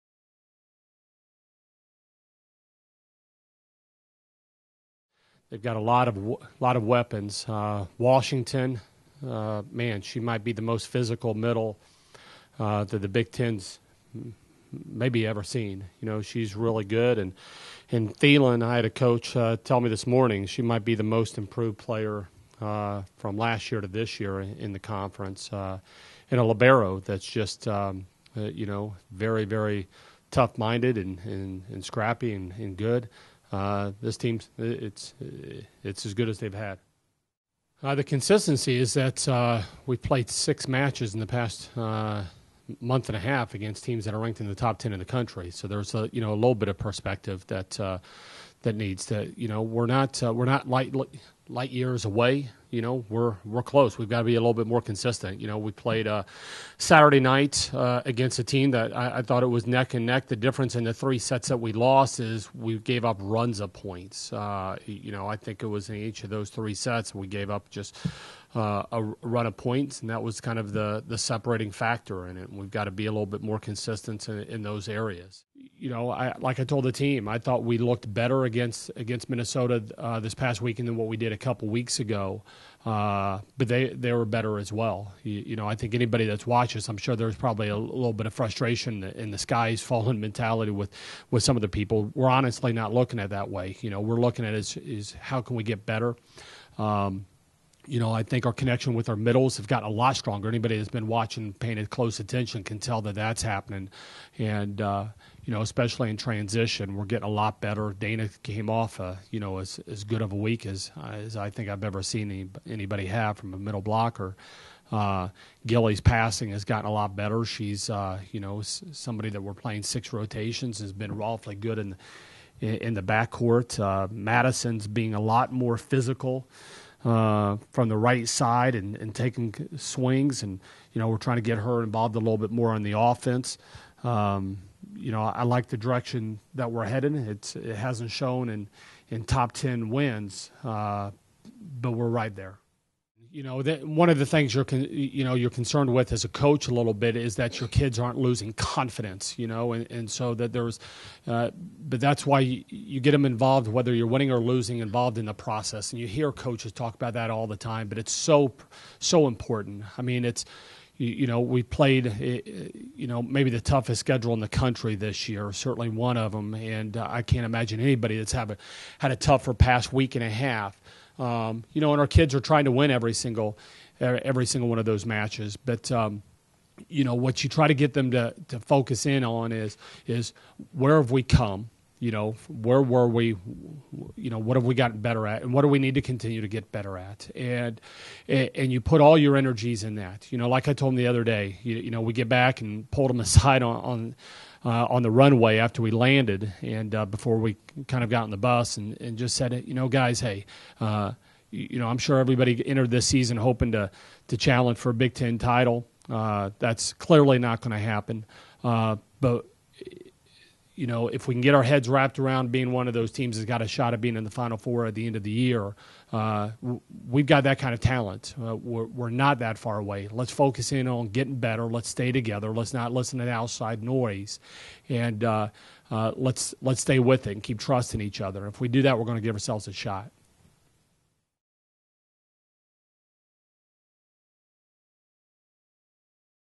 Wisconsin volleyball coach Kelly Sheffield discusses Badgers' progress
Badgers volleyball coach Kelly Sheffield talks about facing top-ranked Penn State, his team's consistency and maintaining confidence.